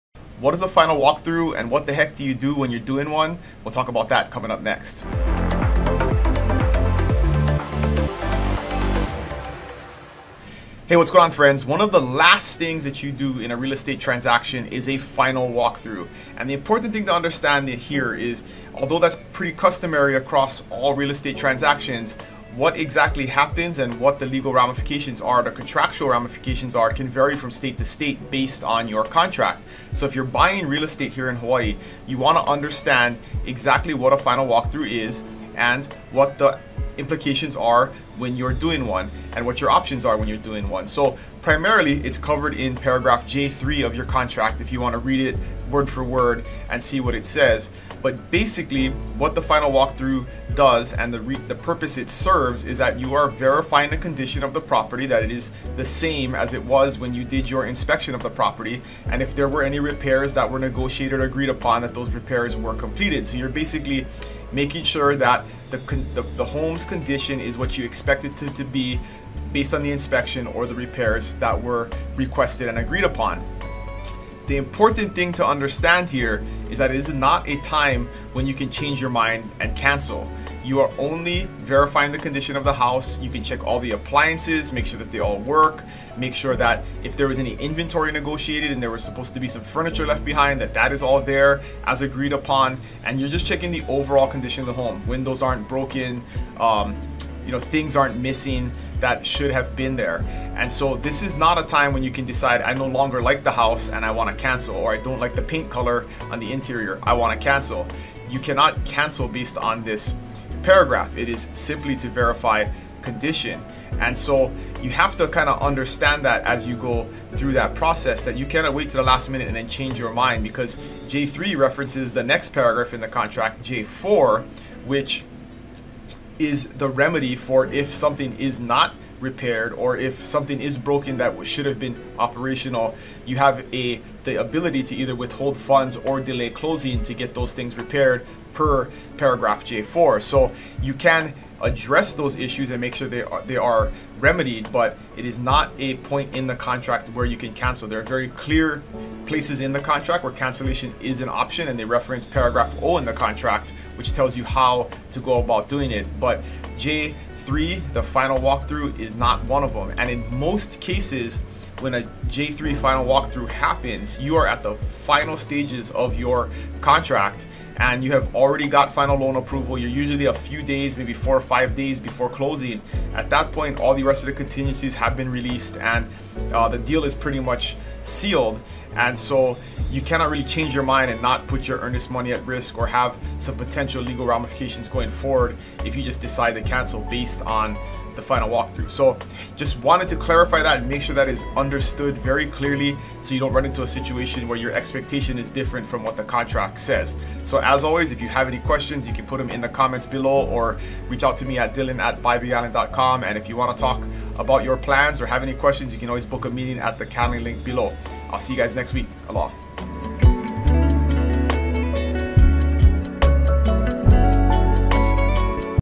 HAWAII DON'T DO THIS During the Final Walkthrough...!🤯 [What To Do Instead 🤫]
What is a finalwalkthrough in realestate ? What happens if you find problems during a final walkthrough? Can you still cancel the deal? In this video, I dylannonaka from hawaii exprealty will share all the important things you NEED TO KNOW when doing a final walkthrough before closing. A FINAL WALKTHROUGH is an essential step in a real estate transaction that typically takes place a few days before closing. It allows the homebuyers to confirm that the property is in the same condition as it was when they agreed to purchase it, and that any agreed-upon repairs or changes have been made. A final walkthrough is crucial, as it allows the home buyer to identify potential issues before closing and ensures that the property is delivered as agreed upon in the contract. Understanding the whole process during this stage is important to set the proper expectations before closing a deal. This will avoid confusion and make a sale's closing smoother without any hassle. Like this content?

🕒TIMESTAMPS🕒
00:00 What is a Final Walkthrough?
00:40 Main reference in understanding the final walkthrough.
00:47 What is the purpose of a final walkthrough?
01:14 Can you still cancel a deal during the final walk through?
01:55 What happens if you find problems during a final walk through?
02:39 What to expect during the final walkthrough?
03:16 Have more questions? Let me know!

👉Are you a Homeowner in Kailua-Kona?
👉 Check out the Most Popular Home for Sale in Waikoloa Today
👉 Check out the Most Expensive Home for Sale in Captain Cook Today